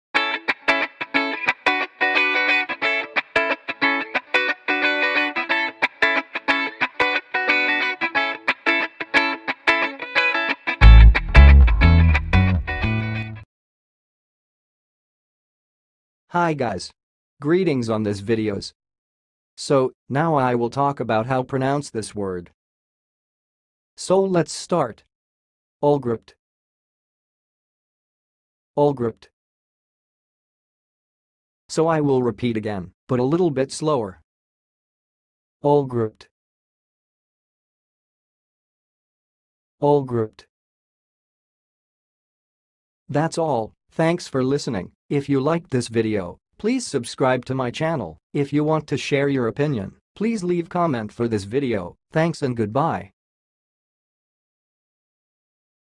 AWLGRIPPED - HOW TO PRONOUNCE IT!?
Watch how to say and pronounce "awlgripped"!